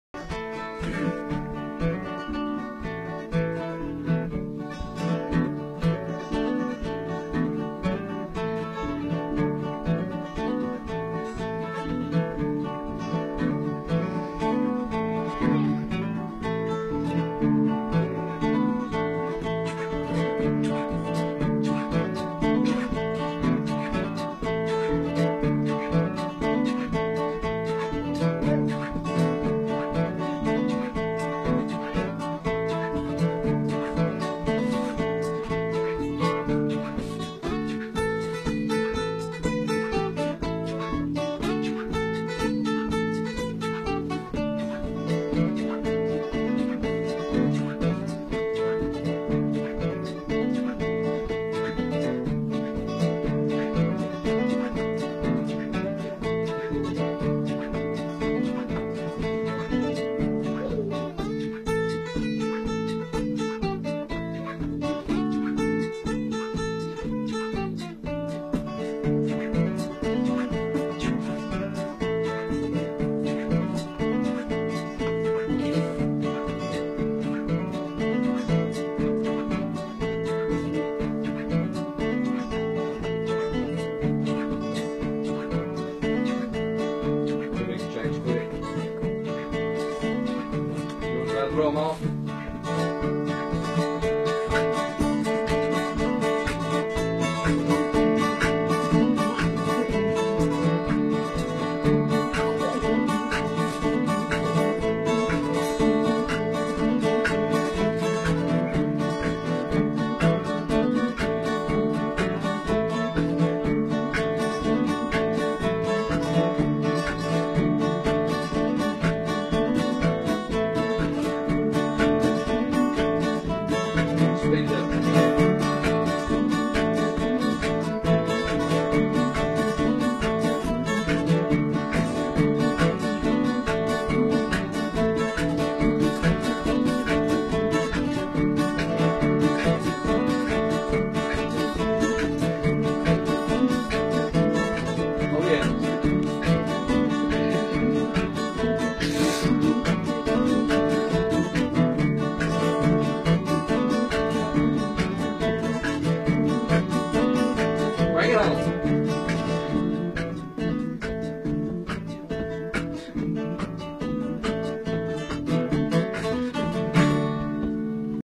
Just Jamming out
Just playing some music with my brother, he's on the bass, and having some beatboxing on the mix, by Alan. Enjoyed it so much.